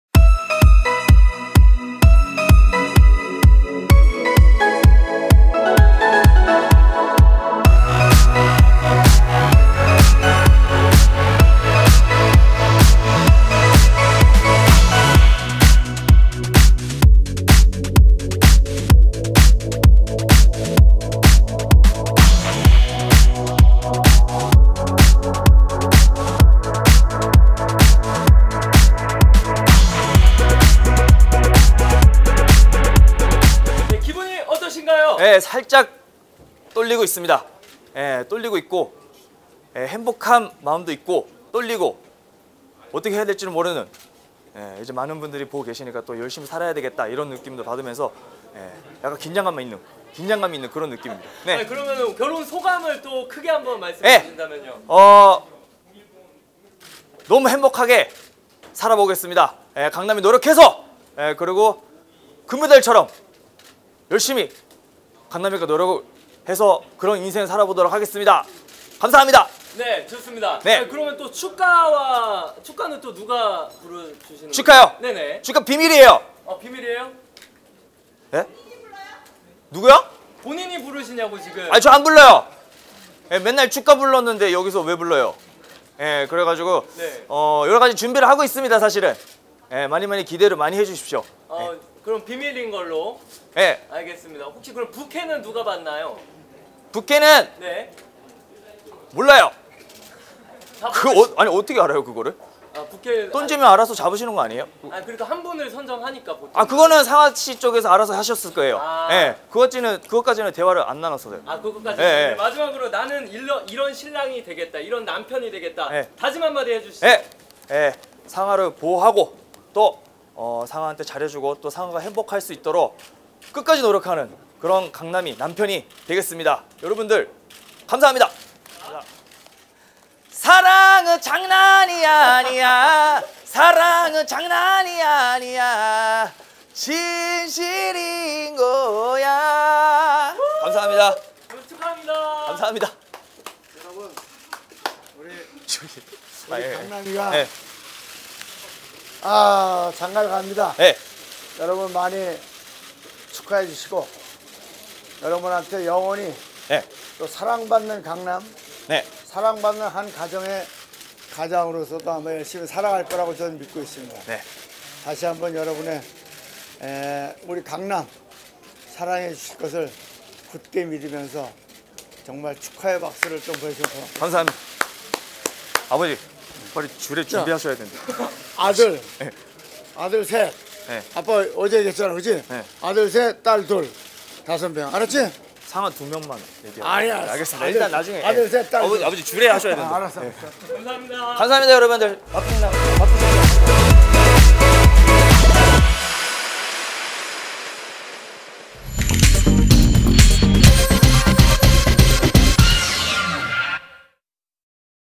[S영상] 강남 ♡ 이상화 결혼식 현장, '언제나처럼 밝고 재밌는 강남의 결혼 소감'
[스타데일리뉴스=소가영 기자] 강남, 이상화가 12일 오후, 서울 광진구 워커힐호텔에서 결혼식을 올렸다.

이날 강남과 이상화의 결혼식에는 광희, 문가비, 장문복 등 함께 방송에 출연했던 동료들이 하객으로 참석했고 강남이 아버지처럼 여기는 태진아가 주례를 맡았다.